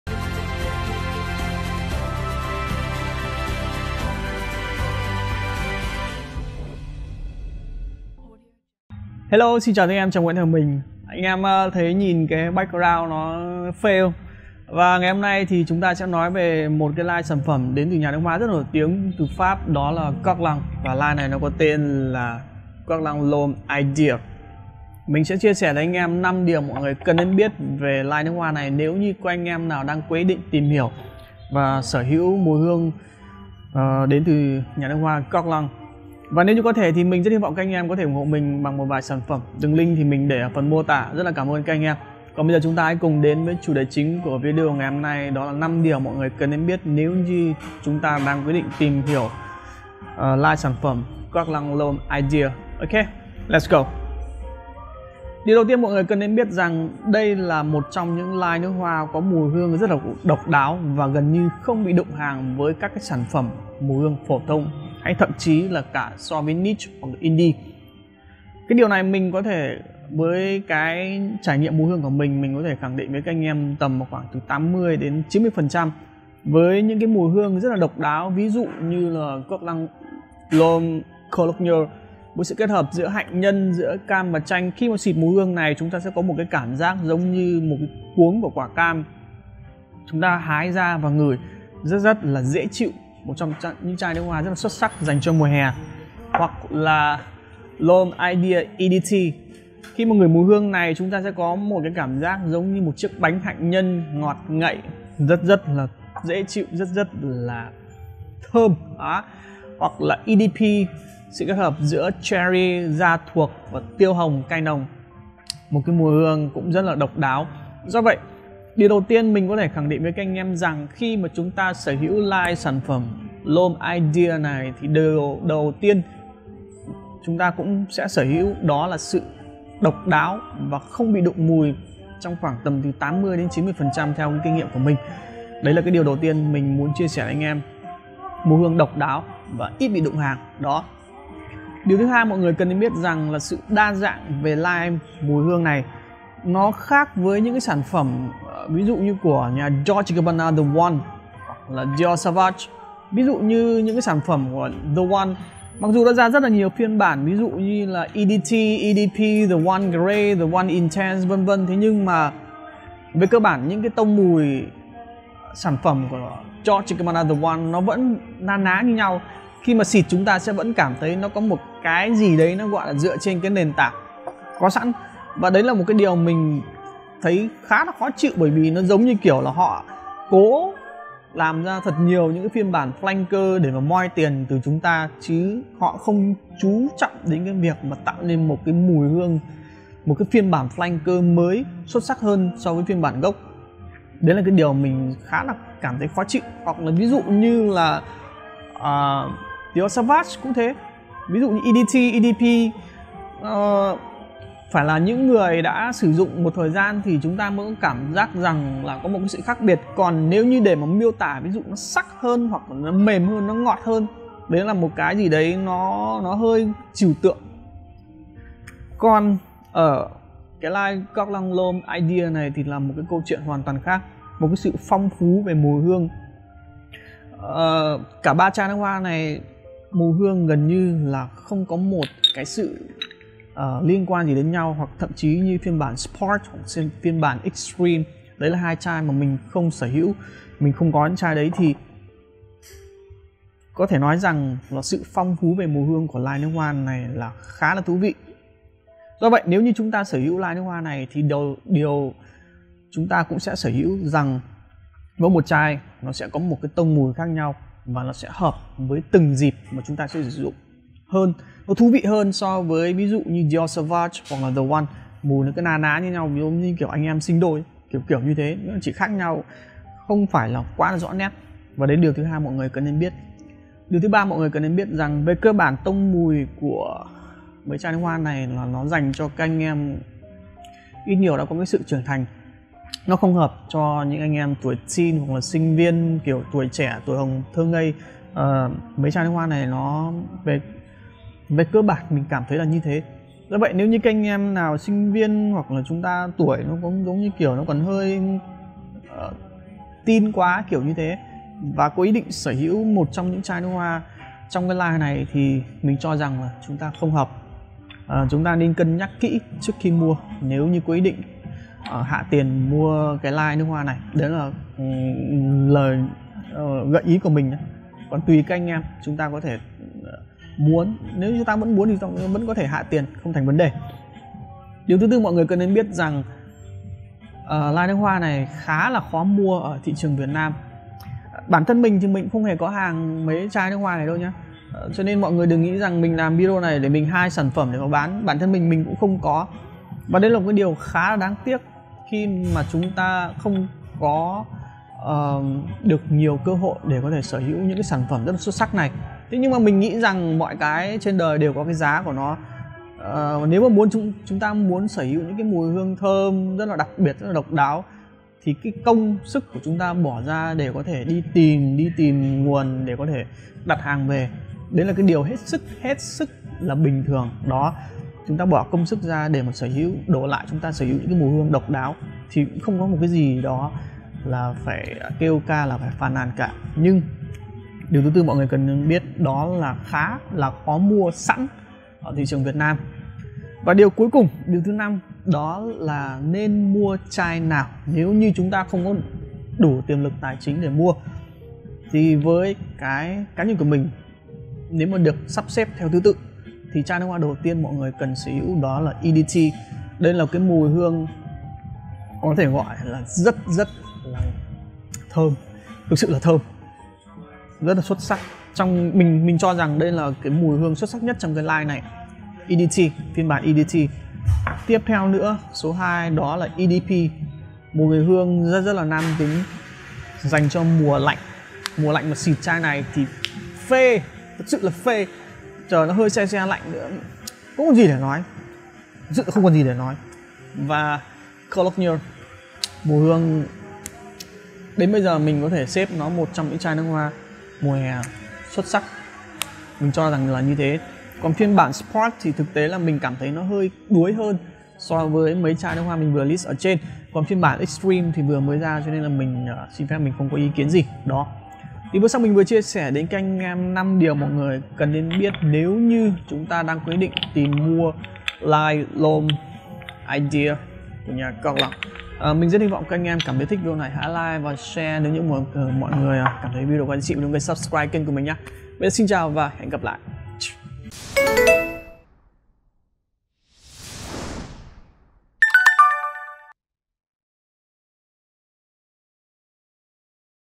NƯỚC HOA GUERLAIN L'HOMME. CHO AI CHƯA BIẾT...
Video này mình chia sẻ a e đánh giá về line Guerlain L'homme Ideal. Một trong những line nước hoa với mùi hương rất xuất sắc & ít đụng hàng